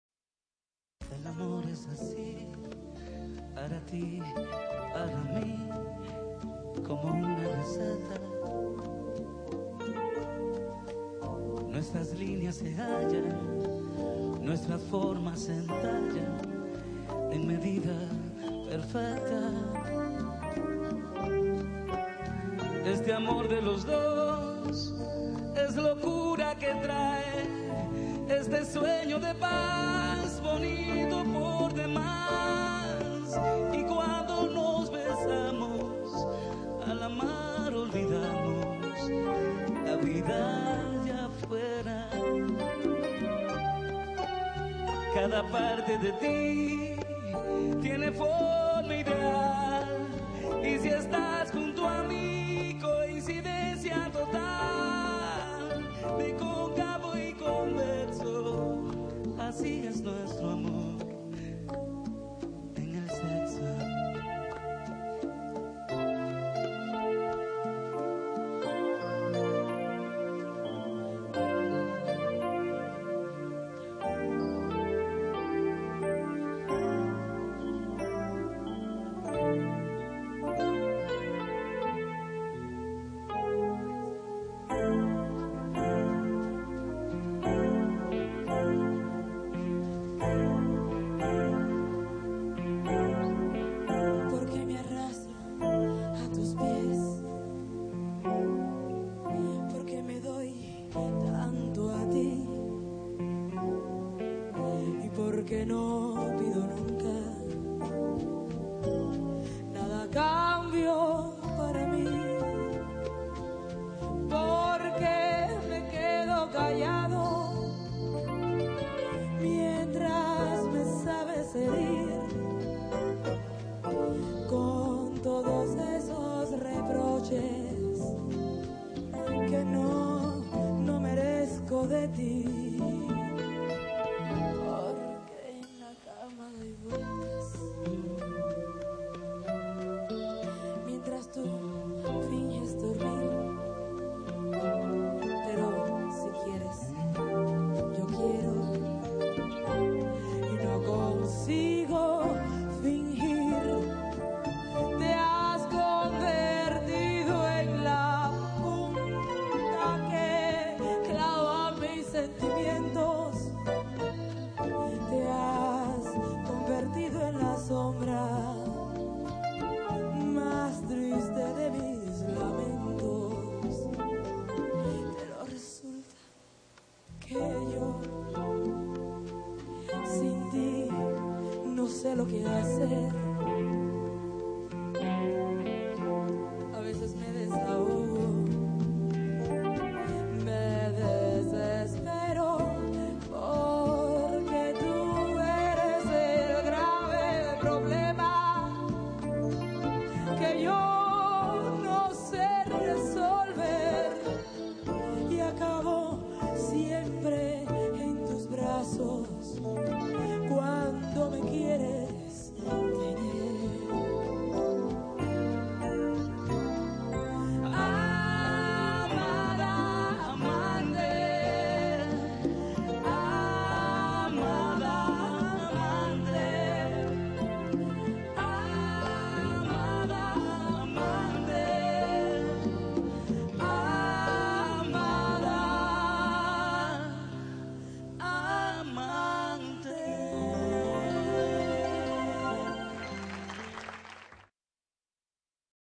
Nostalgia Cubana - Osdalgia y Waldo Mendoza - Pouporrit de Roberto Carlos
Maravillosa interpretacion de los números de Roberto Carlos por Osdalgia y Waldo.